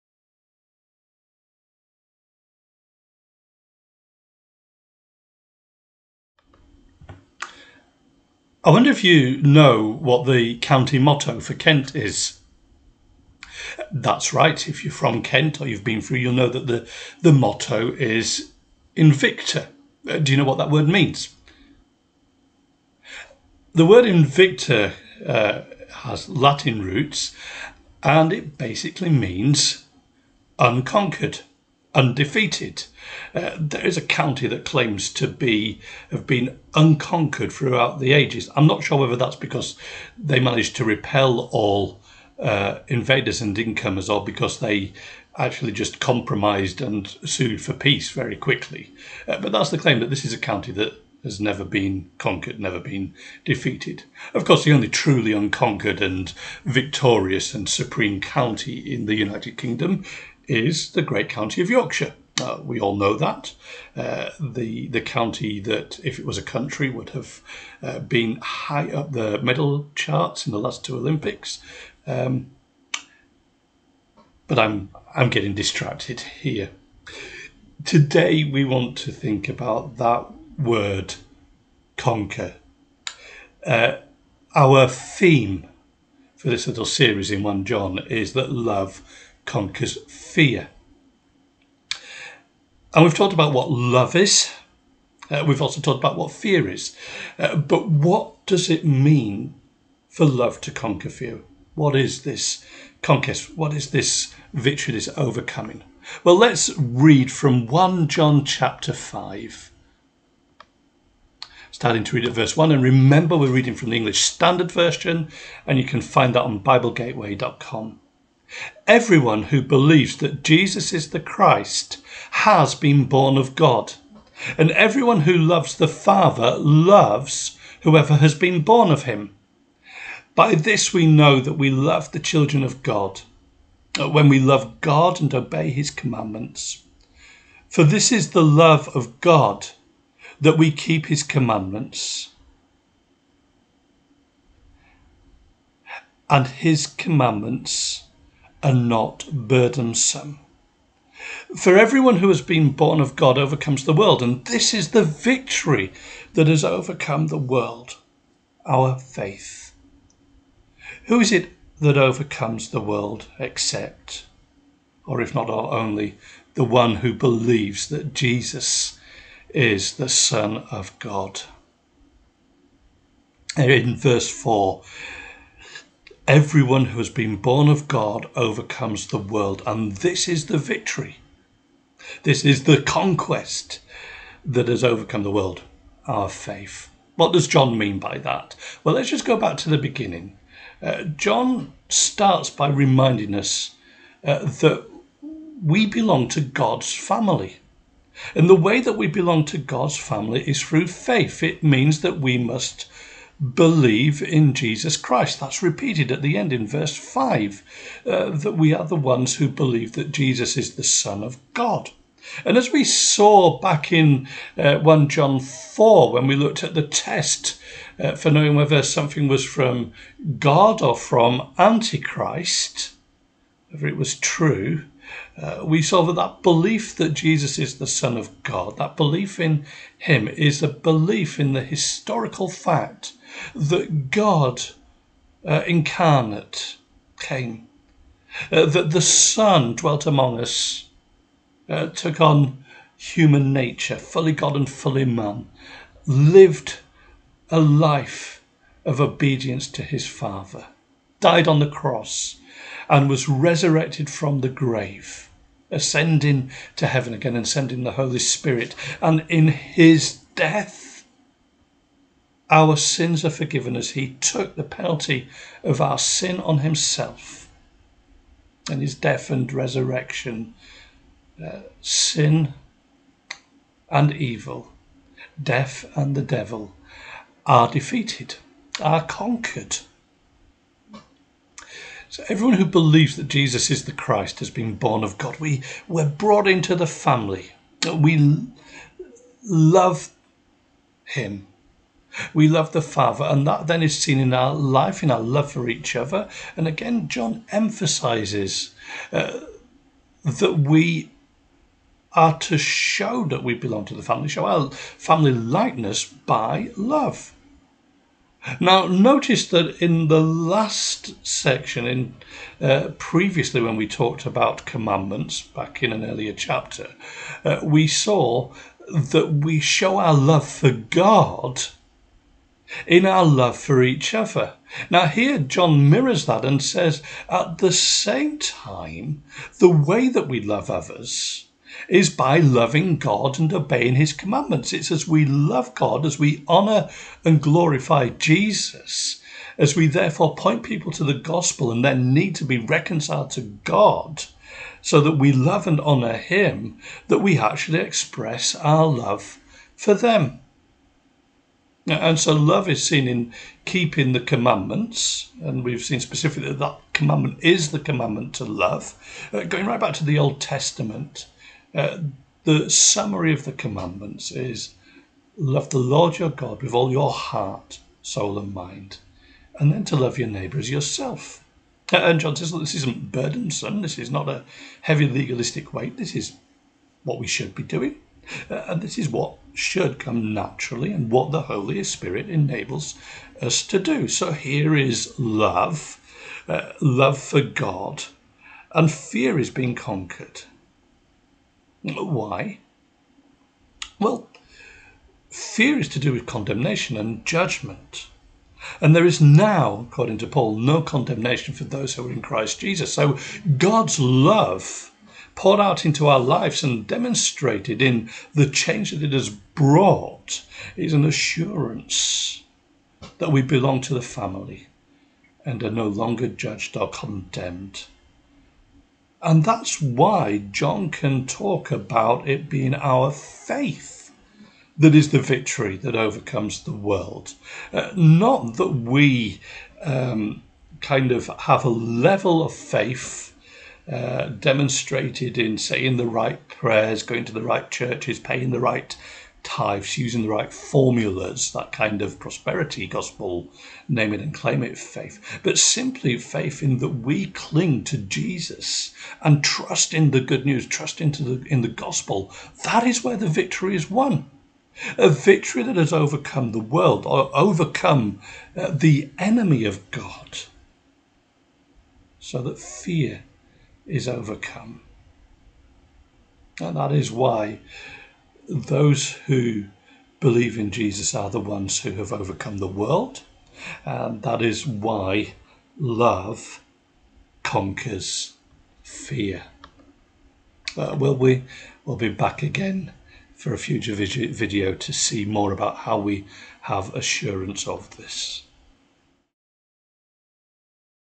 Love Conquers Fear 12 Victory
How is victory won over fear?  1 John 5:1-5